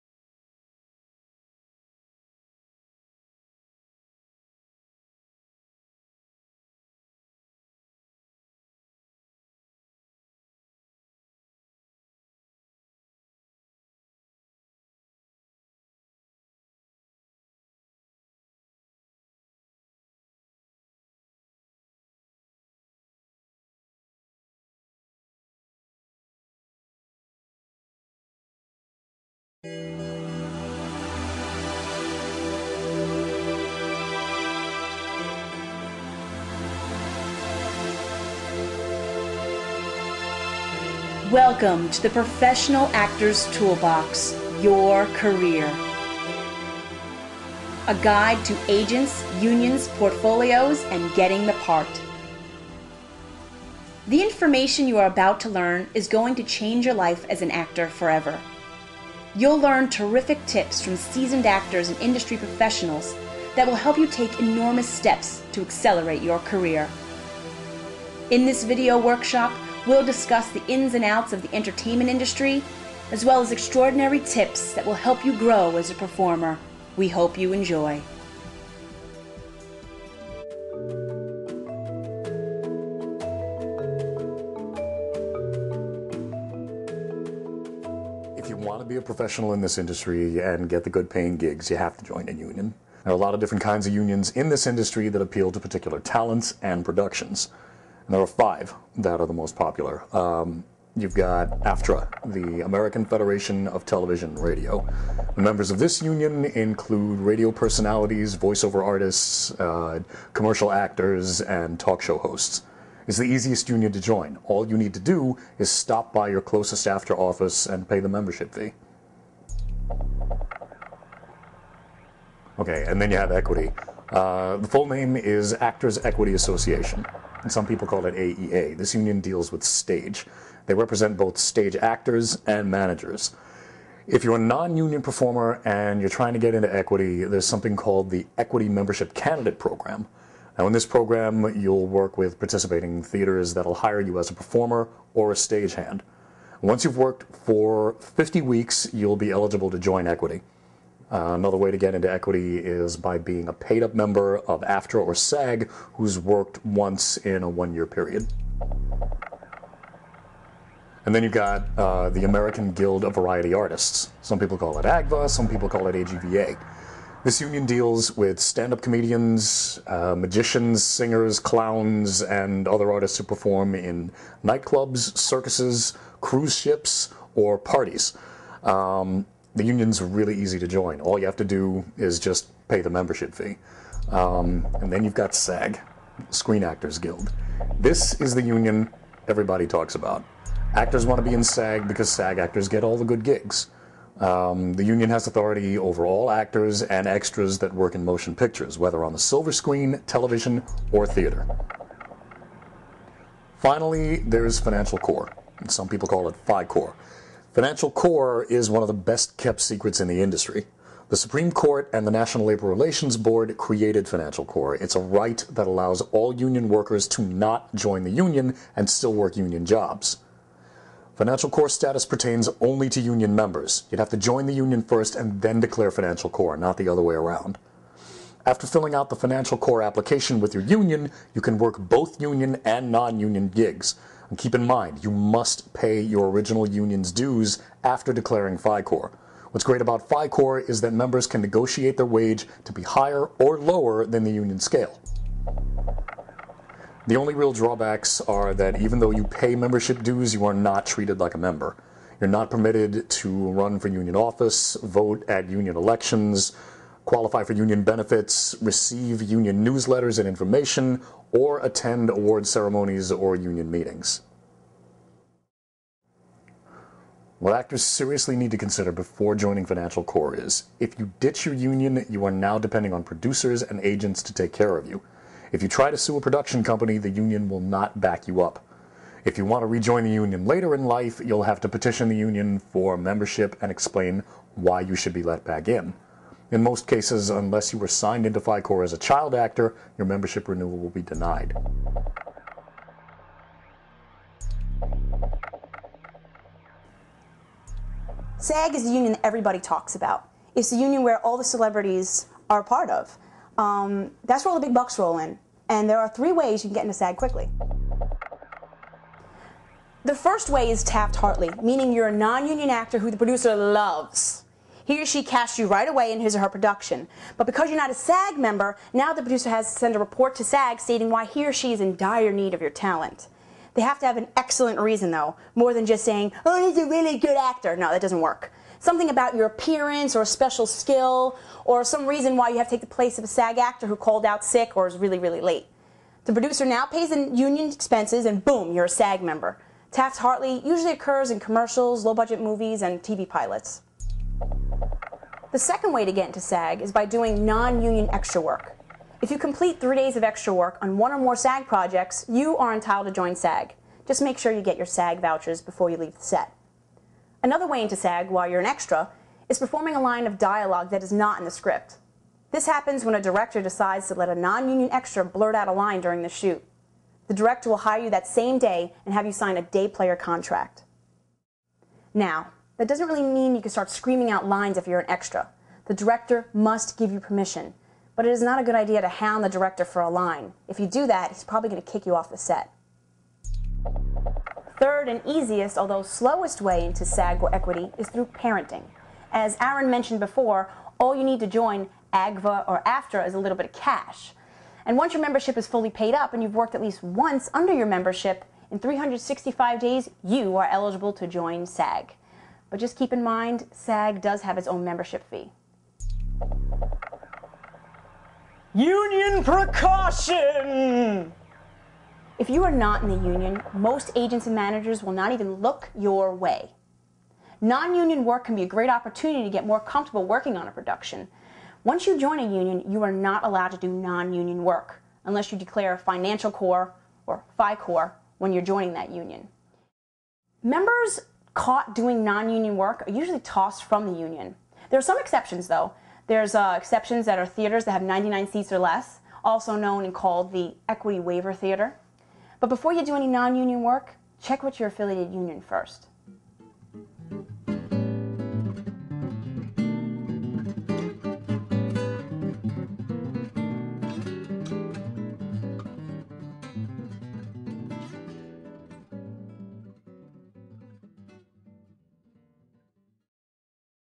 A ) Unions (Part 1 of 11) - ProActorsToolbox.com
Actors: Learn about union jobs, regulations and how to join them easier!